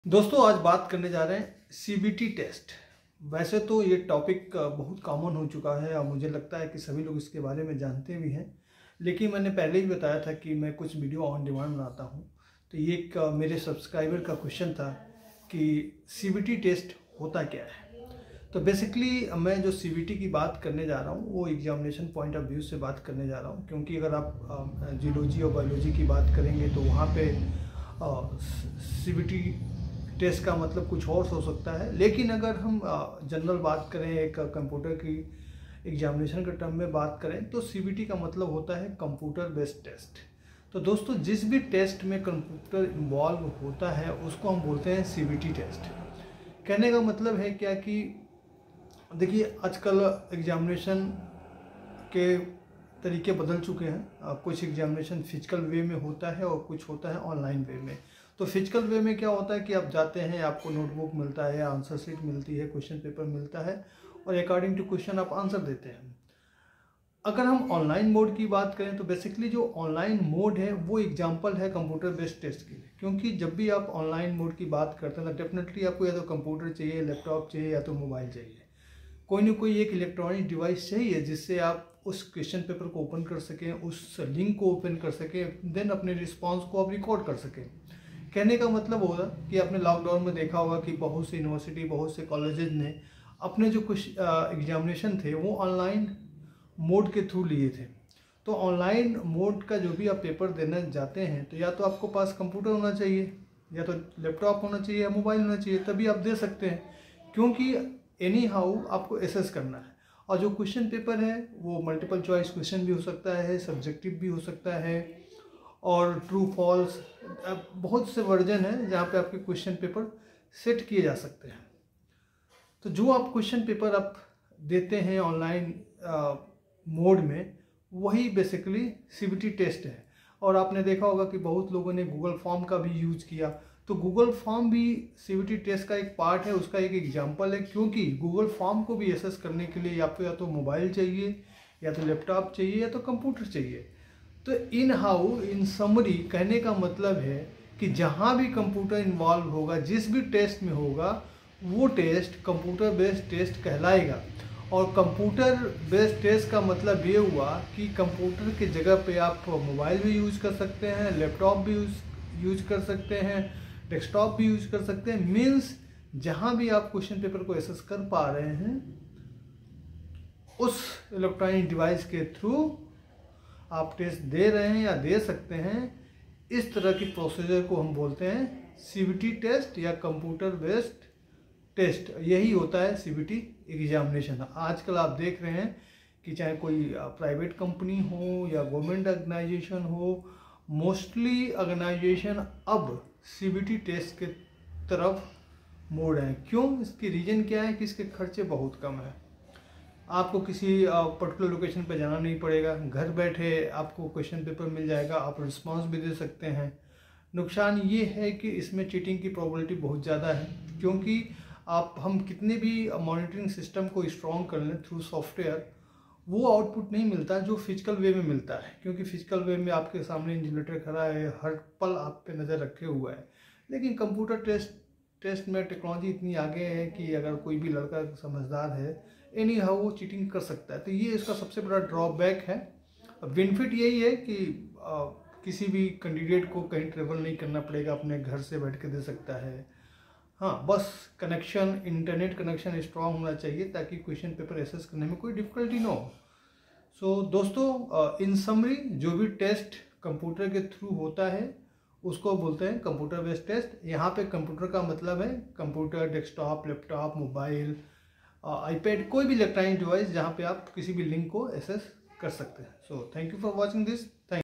What is CBT in Examinations? Why it is in Demand now days ?
However, organizations also faced the brunt of these disruptions. This is possibly why most institutions are opting for a CBT exam in order to minimize the administrative challenge of scheduling a written exam, printing papers, and the laborious task of evaluating written answers.